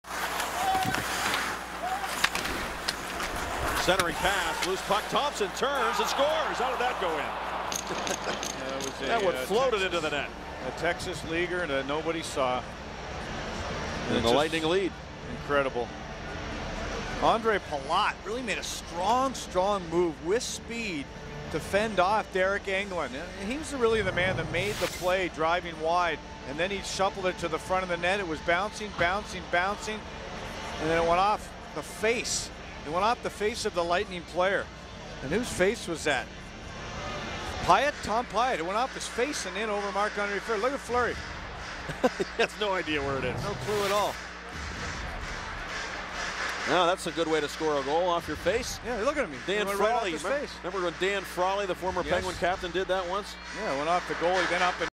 Tom Pyatt scores a goal with his face - Penguins vs Lightning - March 4th 2013
Love the slow mo replay with Fleury just sitting there.